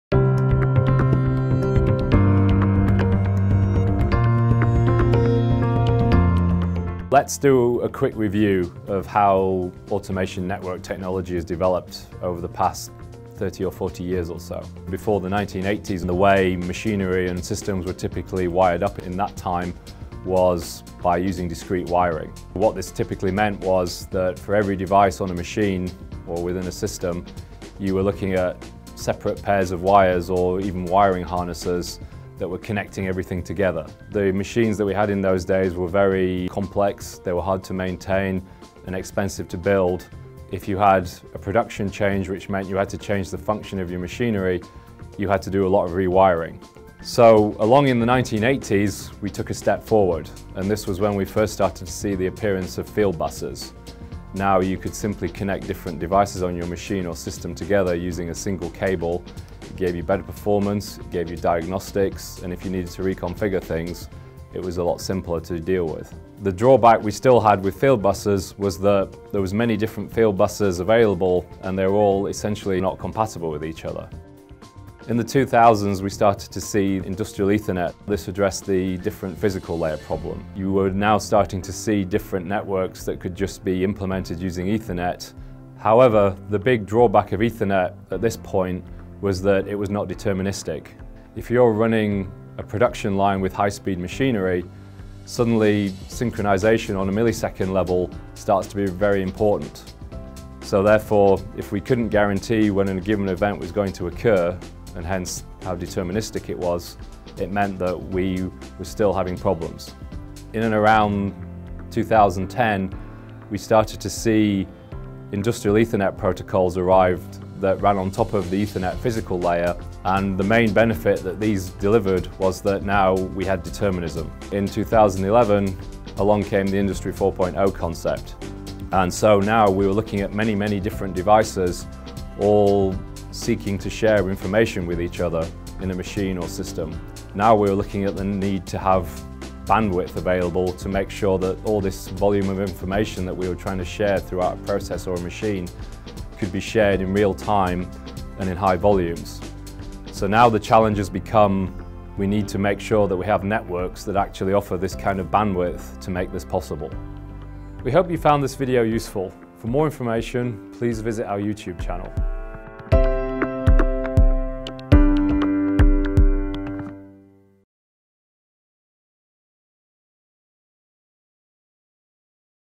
Industry 4.0 - Past, Present and Future: Part 1 The History of Industry 4.0
Part 1 of this 5 part series, sees John Browett, General Manager for CLPA Europe, explain how automation technology has developed through the decades and how Industry 4.0 will depend on increased bandwidth moving forward.

Postfach 10 12 17, 40832 Ratingen, Germany